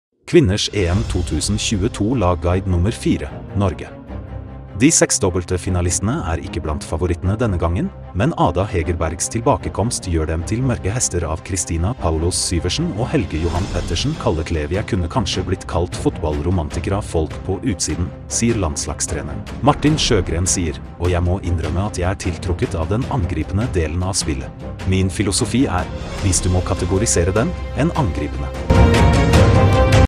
Kvinners EM 2022 lagguide nr. 4: Norge :norwagen
Kvinners EM 2022 lagguide nr. 4: Norge
 De seksdobbelte finalistene er ikke blant favorittene denne gangen, men Ada Hegerbergs tilbakekomst gjør dem til mørke hester av Christina Paulos Syversen og Helge Johan Pettersen Kalleklev Jeg kunne kanskje blitt kalt fotballromantiker av folk på utsiden, sier landslagstreneren. Martin Sjögren sier, "og jeg må innrømme at jeg er tiltrukket av den angripende delen av spillet. Min filosofi er, hvis du må kategorisere den, en angripende
Håndball-EM 2022